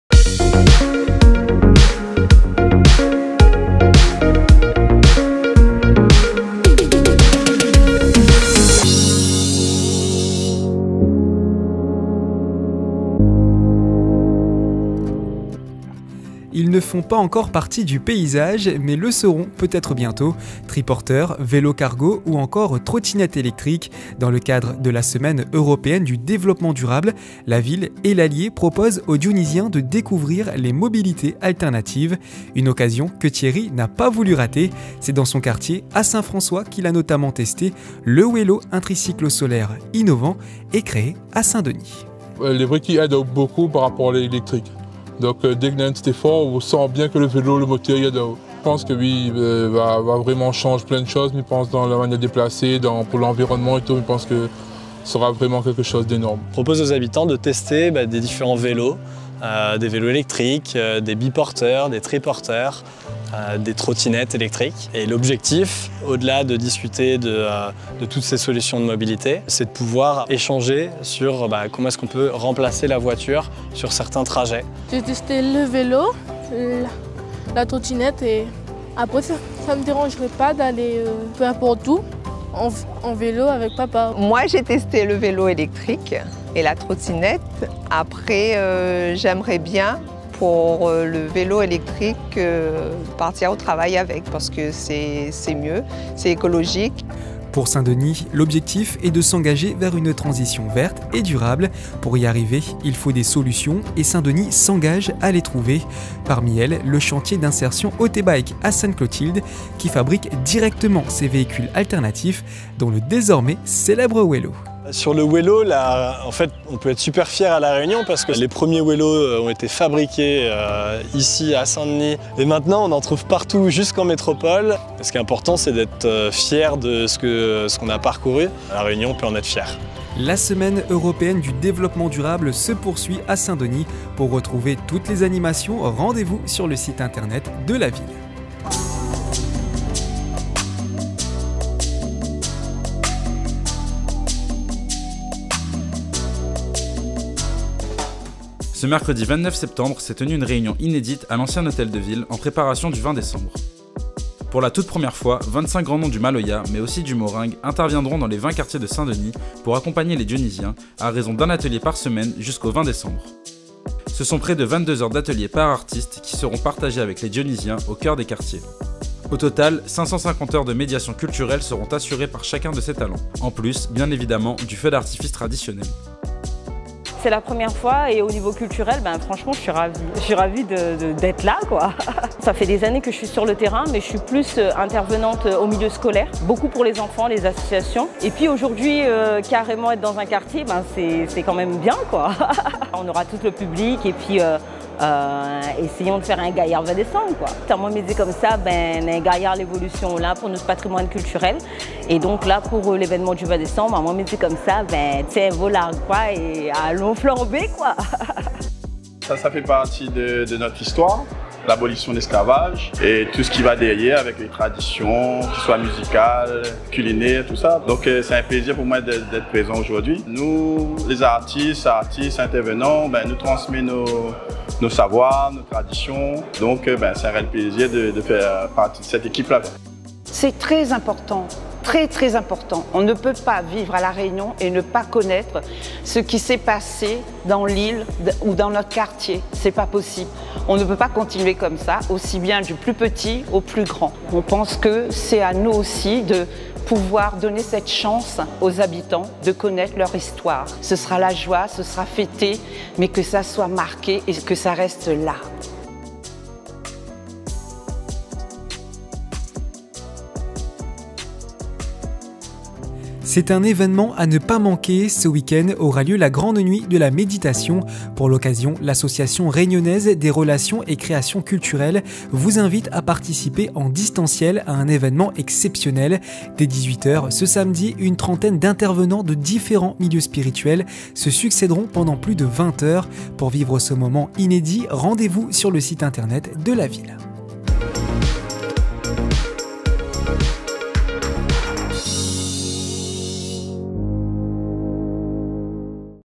Dionycité | Découvrez les mobilités alternatives / 25 maloyèr & moringèr dann kartié
Et si on apprenait à circuler autrement ? Triporteur à assistance électrique solaire WELLO, vélos ou encore trottinettes électriques... Au côté de l'assocation ALIE, les habitants de Saint-François ont pu experimentés diverses solutions de transports alternatives à la voiture !

Dès le 1er octobre, ce ne sont pas moins de 25 grands noms du Maloya et du Moring qui viendront à votre rencontre, une fois par semaine, dans les quartiers pour partager savoir et tradition à travers un atelier de médiation culturelle. Une première à La Réunion en préparation du 20 désanm !